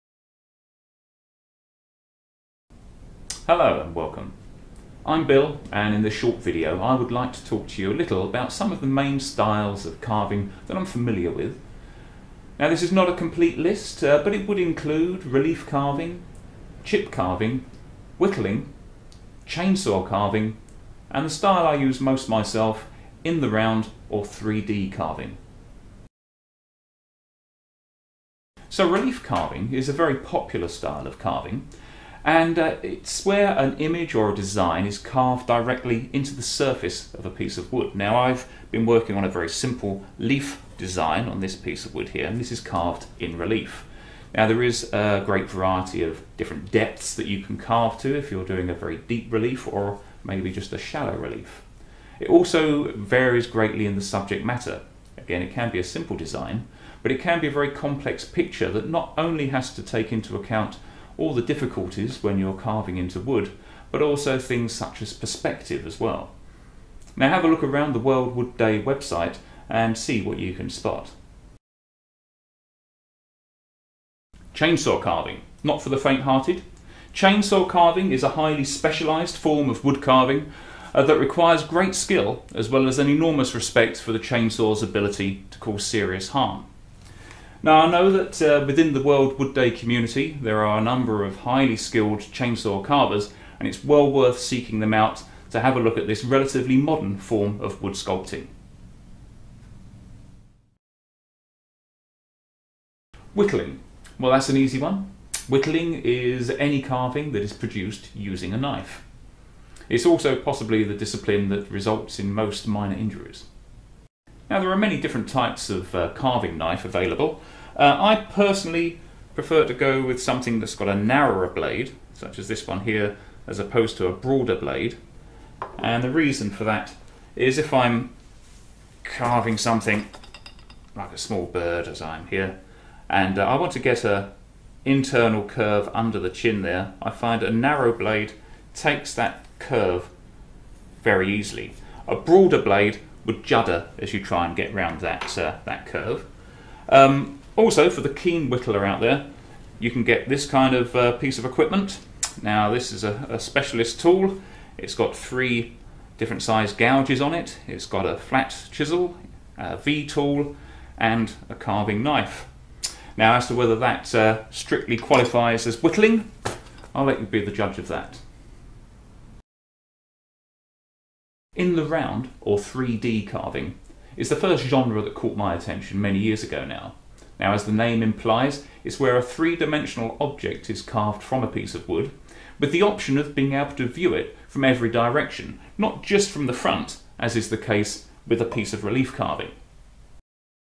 WWD 2021 Woodcarving - Basic Demonstration feat. Bill Prickett: Styles of Woodcarving
The British artist Bill Prickett briefly talks about styles of woodcarving that he's familiar with.